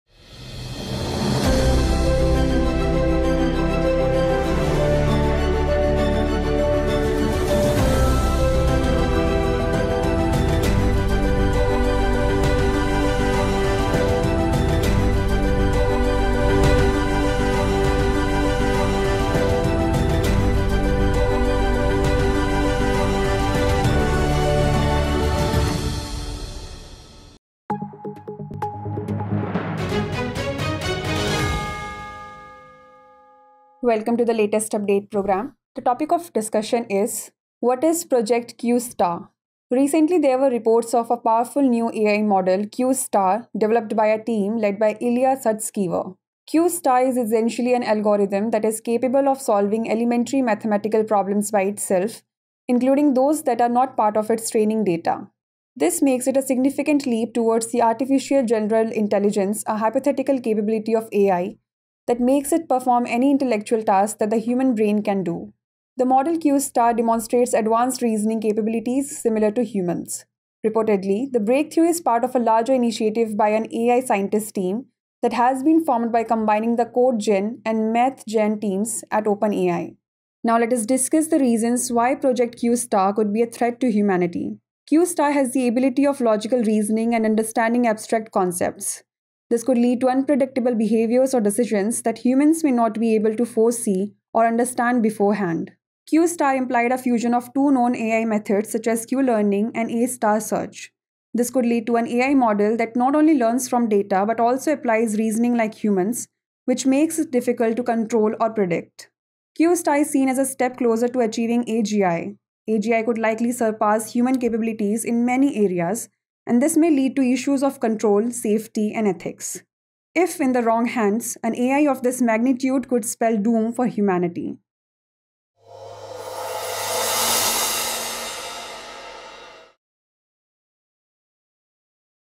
What Is Project Q* ? | Latest update | Drishti IAS English
OpenAI recently faced internal turmoil with the dismissal of Sam Altman and staff threats to resign, amidst concerns over a new AI model, Q*, which represents a significant step towards Artificial General Intelligence but raises safety and ethical issues.